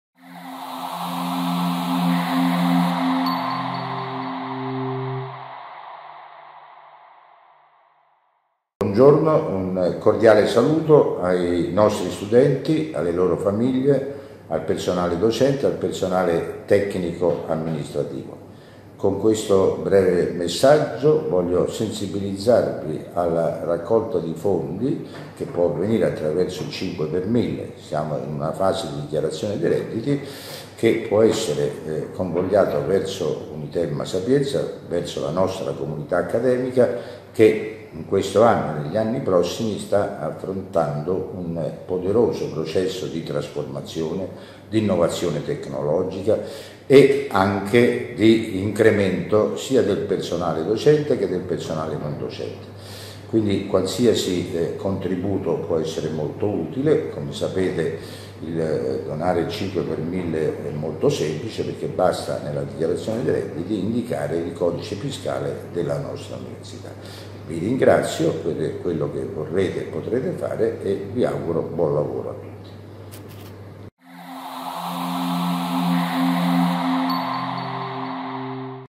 Dona il 5x1000 a UnitelmaSapienza
Il 5x1000 è una quota del gettito fiscale IRPEF delle persone fisiche con l’obiettivo di sostenere le organizzazioni senza scopo di lucro a finalità sociale. Il 5x1000 è una scelta volontaria, non costa nulla e non è un tributo aggiuntivo.

Il Magnifico Rettore prof. Antonello Folco Biagini illustra tutte le buone ragioni per donare il 5 per mille al nostro Ateneo. A tale scopo è necessario firmare e apporre il codice fiscale dell'Università nell'apposito riquadro “Finanziamento della ricerca e dell’Università” sui moduli per la dichiarazione dei redditi.

Il codice fiscale di Unitelma Sapienza è: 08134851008.